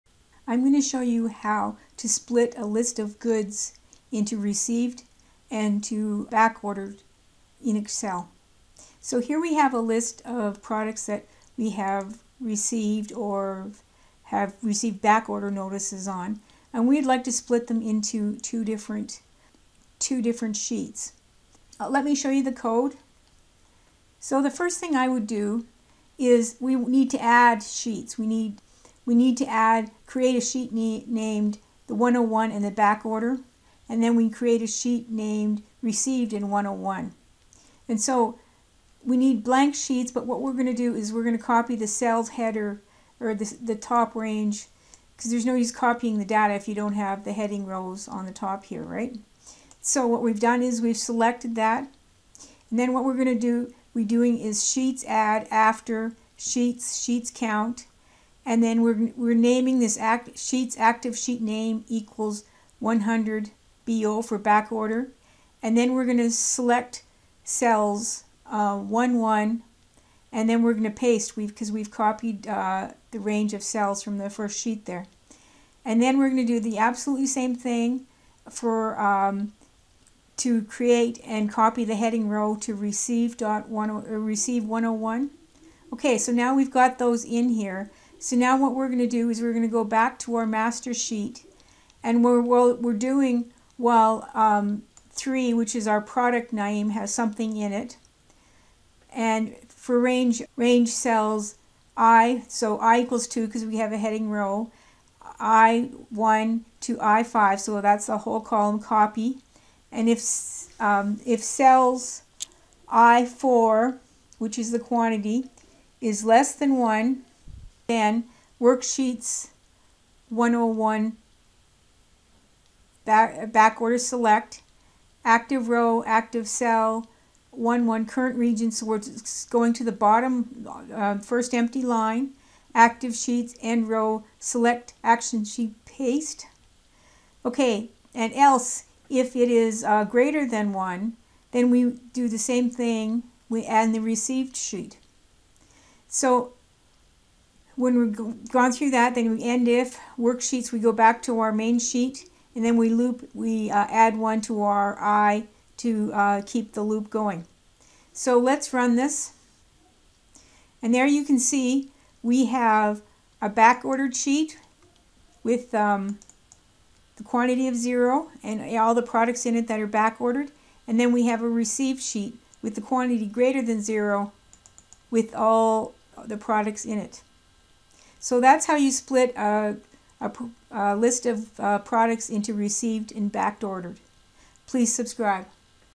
Divide products into Back ordered and received in Excel with VBA
Warehouse inventory levels. Split a list of products into Back ordered and received in Excel.  VBA code included
Add sheets to a workbook.  Copy lines based on their value to separate sheets.

Sub addsheets()
Dim erow As Long

Worksheets("101").Select
Range(Cells(1, 1), Cells(1, 5)).Copy

'create a sheet named 101bo
    Sheets.Add after:=Sheets(Sheets.Count)
    Sheets(ActiveSheet.Name).Name = "101bo"
    ActiveSheet.Cells(1, 1).Select
    ActiveSheet.Paste
'create a sheet named rec101
    Sheets.Add after:=Sheets(Sheets.Count)
    Sheets(ActiveSheet.Name).Name = "rec101"
    ActiveSheet.Cells(1, 1).Select
    ActiveSheet.Paste

Worksheets("101").Select

i = 2
Do While Cells(i, 3) (the symbole for not equal) ""
Range(Cells(i, 1), Cells(i, 5)).Copy
    If Cells(i, 4) (symbol for less than) 1 Then

        Worksheets("101bo").Select
        erow = ActiveSheet.Cells(1, 1).CurrentRegion.Rows.Count + 1
        ActiveSheet.Cells(erow, 1).Select
        ActiveSheet.Paste
    Else
        Worksheets("rec101").Select
         ActiveSheet.Cells(erow, 1).Select
         ActiveSheet.Paste

    End If
    Worksheets("101").Select
i = i + 1
Loop

End Sub

I am able to provide online help on your computer for a reasonable rate.

MY videos are all created on Camtasia.